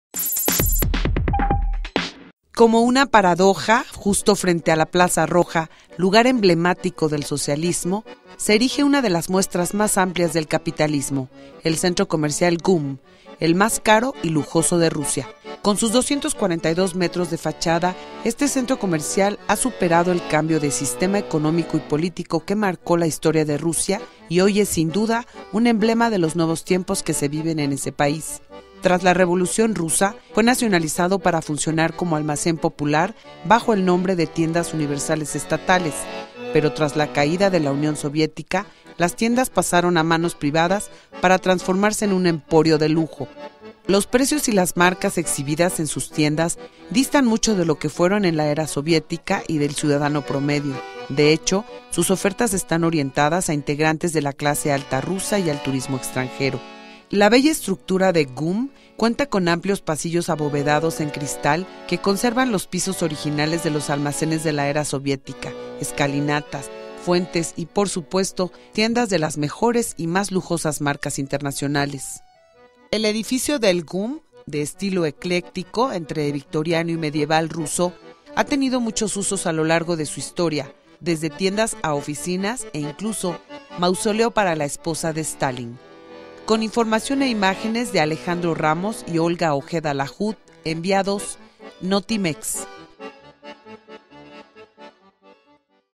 En la Plaza Roja se alza uno de los emblemas del Capitalismo
Frente al recinto más importante de la era socialista en Moscú, se encuentra en Centro Comercial más caro y lujoso de Rusia